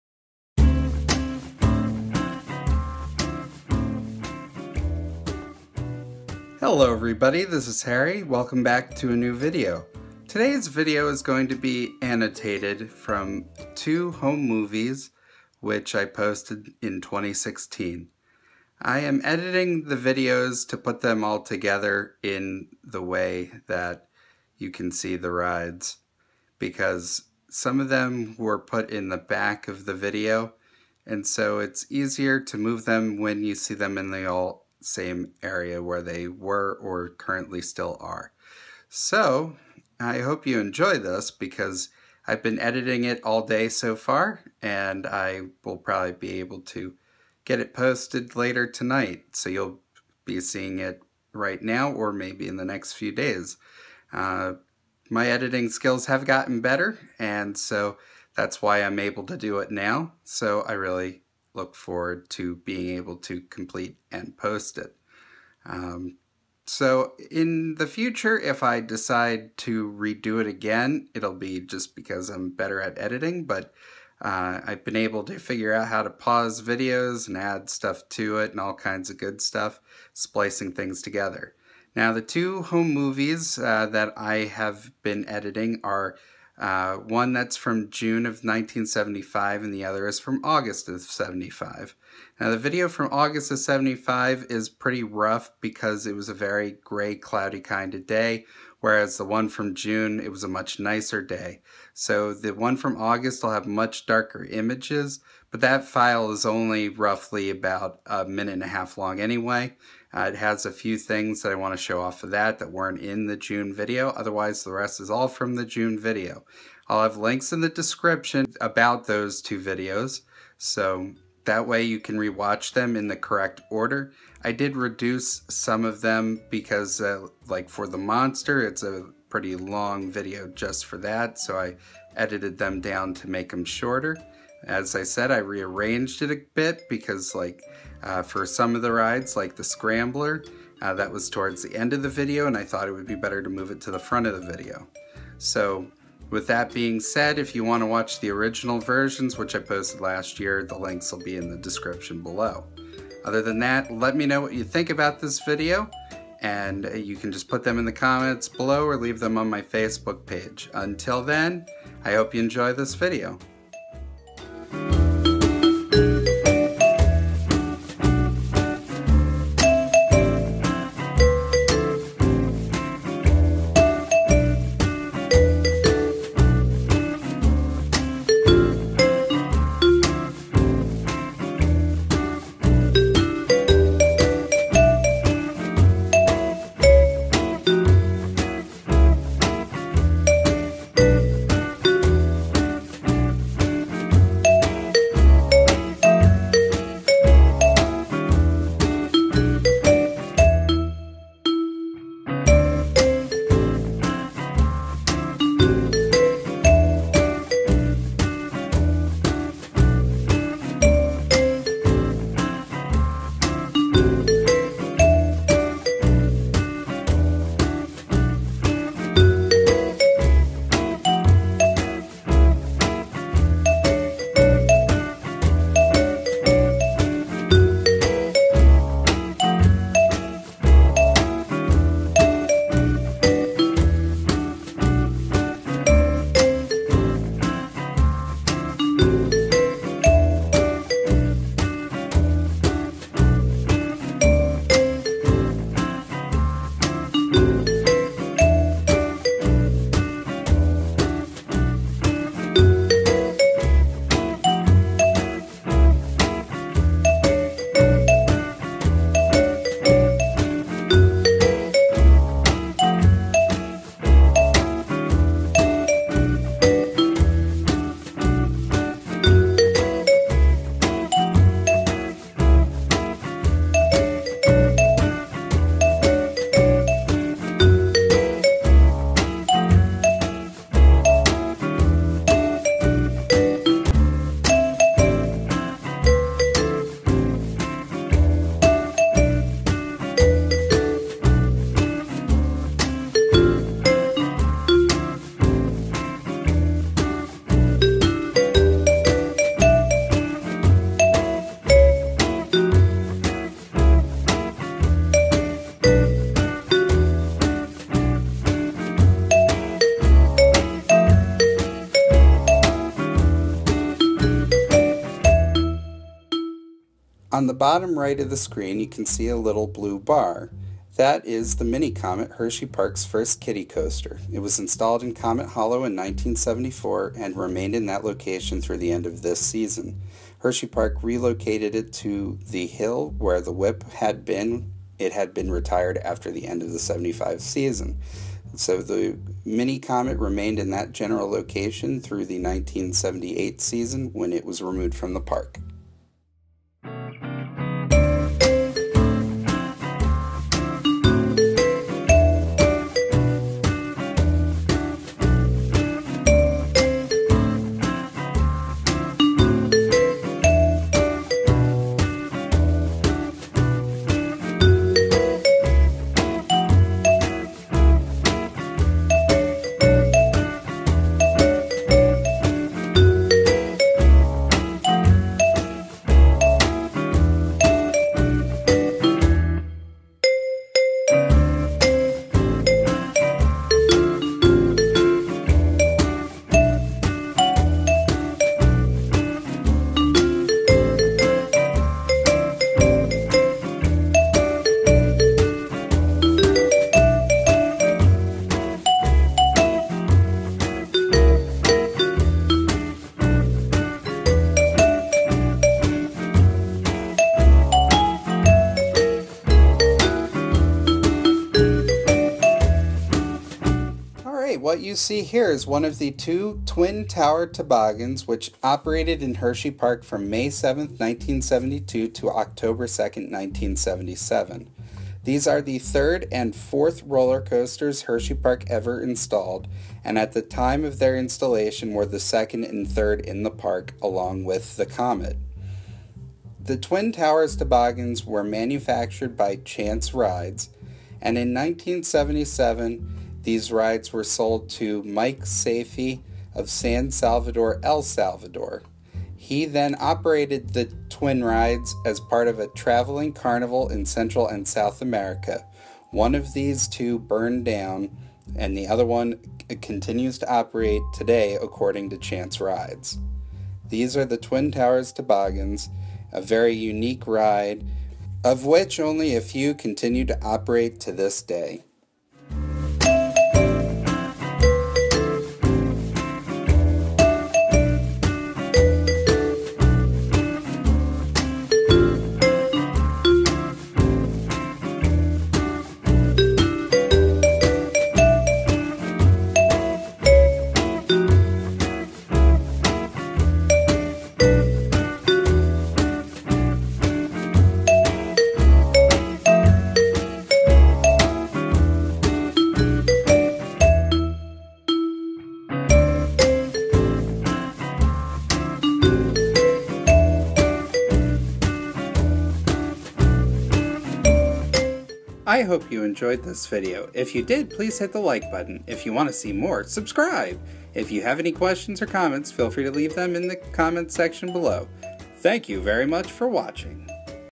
Hersheypark in the Summer of 75
I have annotated two home movies of Hersheypark from 1975. The first video is from June 1975 while the second video is from August 1975. The videos show a number of rides. Some rides are labeled while others are labeled and have additional information.